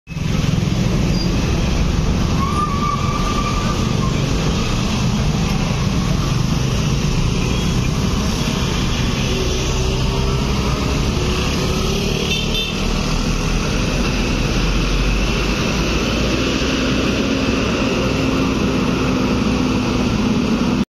Librando Trans 454
Zhongtong LCK6118H "Elegance"
Airconditioned
Cebu City - Bato via Barili (Cebu - Badian)
U0 W993
July 9, 2022 (Alert Level 1 Status)

Like and Follow our Official Cebu Page:

JB BUS SPOTTER AND ENTHUSIAST - Cebu

Device Used on the Videos: Realme GT Master